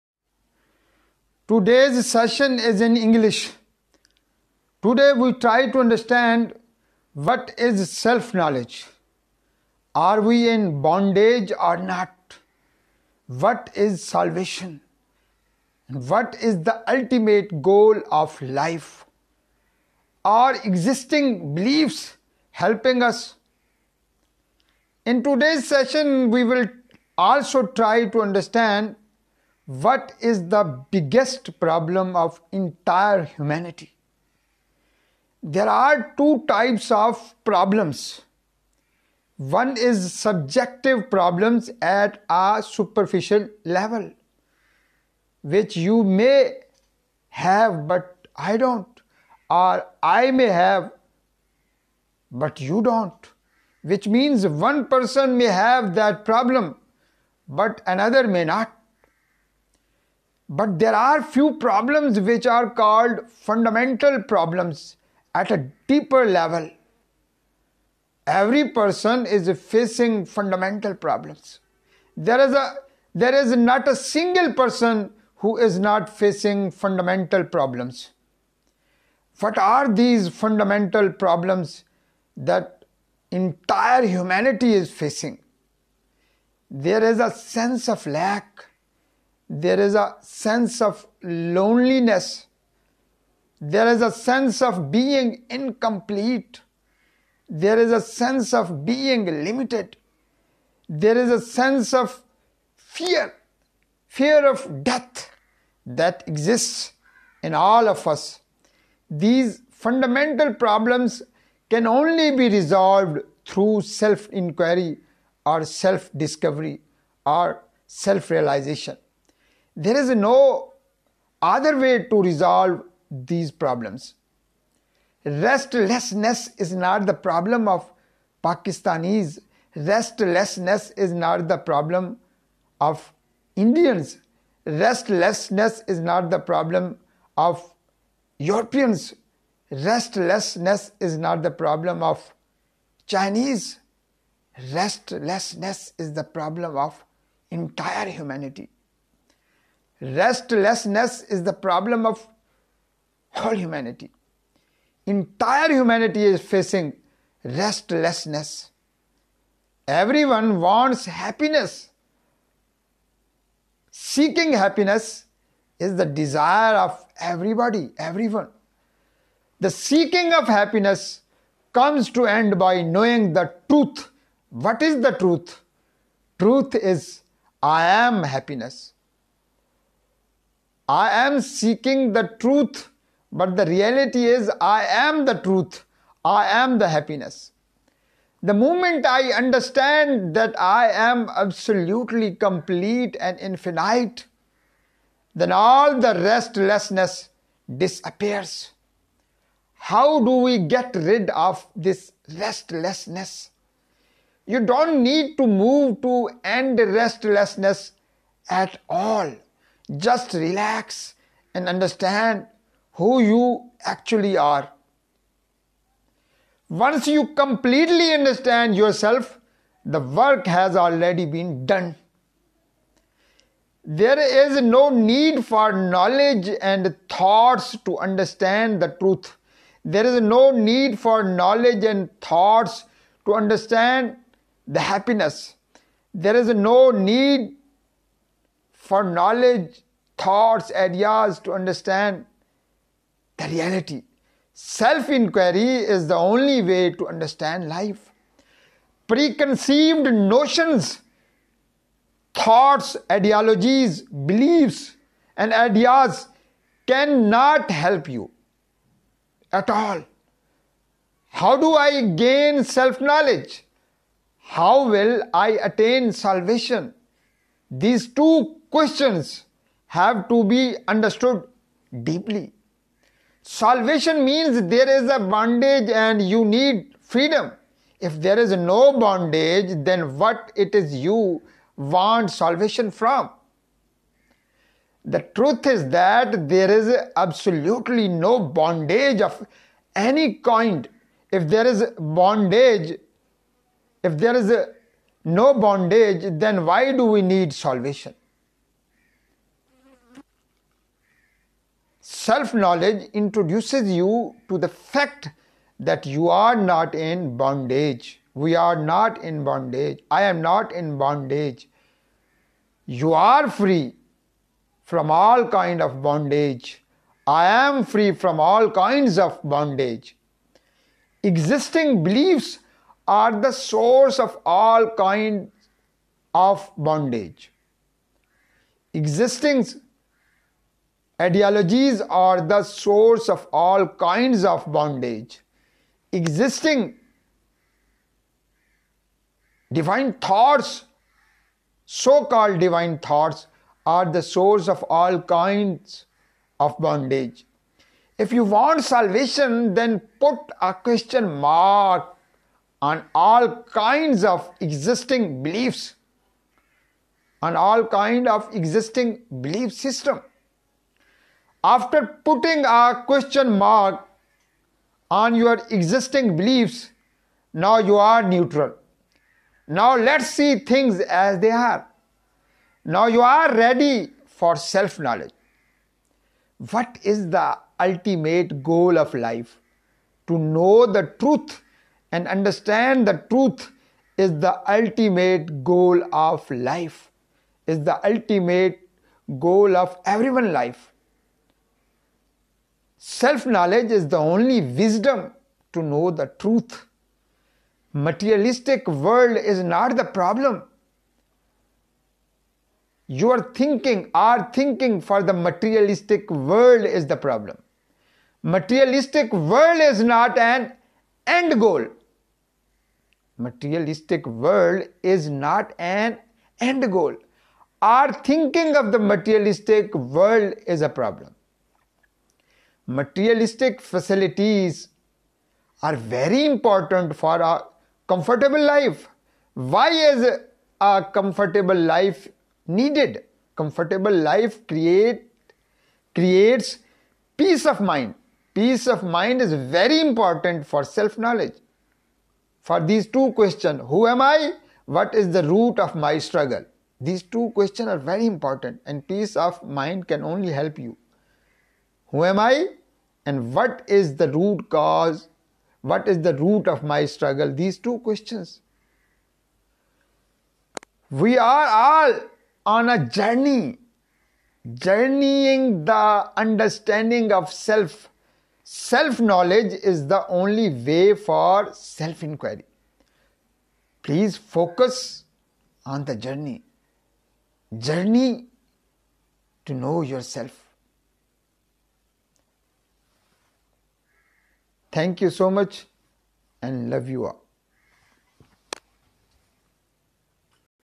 Increase your self-awareness or Self Knowledge | Ajmal Shabbir | House Of Wisdom | English
SELF KNOWLEDGE

Today's session is in English, today we try to understand what is self knowledge? Are we in BONDAGE or not? What is SALVATION?
What is the ultimate goal of life? Are EXISTING BELIEFS helping us understand life or not?

In today's session we will also try to understand what is the biggest problem of ENTIRE HUMANITY?

There are two types of problems ,one is subjective problems at a superficial level, which you may have but I don't, or I may have but you don't, which means one person may have that problem but another may not.

But there are few problems which are called fundamental problems at a deeper level . Every person is facing fundamental problems.
There is not a single person who is not facing fundamental problems.

What are these fundamental problems that whole humanity is facing?

There is a sense of lack,a sense of loneliness ,  a sense of being incomplete, a sense of being limited, a sense of fear, fear of death,  that exists in all of us. These fundamental problems can only be resolved through self inquiry or self discovery. There is no other way to resolve these problems.

Restlessness is not the problem of Pakistanis, restlessness is the problem of humanity. Entire humanity is facing restlessness. Everyone wants happiness, seeking happiness is the desire of everyone.

The seeking of happiness comes to end by knowing the truth. What is the truth? Truth is I am happy.

I am seeking the truth but the reality is I am the truth.
 The moment I understand that I am absolutely complete and infinite , then all the restlessness disappears.

How do we get rid of this restlessness?

You do not need to move to end restlessness at all ,just relax and understand who you actually are?

Once you completely understand yourself, the work has already been done.

There is no need for knowledge and thoughts to understand the truth. Self Inquiry is the only way to understand life. Preconceived notions ,thoughts ,ideologies ,beliefs and ideas can not help you.

How do I gain self knowledge? How will I attain salvation? These two questions have to be understood deeply.

Salvation means there is  a bondage and you need freedom. If there is NO  bondage than what it is you want salvation from?

The truth is that there is absolutely no bondage of any kind ,if there is bondage then why do we need salvation?

Self knowledge introduces you to the fact that you are not in bondage ,you are free from all kinds of bondage.

Existing beliefs are the source of all kinds of bondage. If you want salvation then put a question mark on all kinds of existing beliefs?

After putting a question mark on your existing beliefs ,now you are neutral.

Now let's see things as they are . Now you are ready for self knowledge.

What is the ultimate goal of life ,to know the truth and understand the truth is the ultimate goal of life.
Self Knowledge is the only wisdom to know the truth.

Materialistic world is not the problem, your thinking for the materialistic world is the problem. Materialistic world is not an end goal, our thinking of the materialistic world is a problem.

Materialistic facilities are very important for a comfortable life. Why  is a comfortable life needed? Comfortable life creates peace of mind. Peace of mind is very important for self knowledge
What is the root of my struggle?

We are all on a journey, journeying the understanding of self, self knowledge is the only way for self inquiry. Please focus on the journey ,journey to know yourself .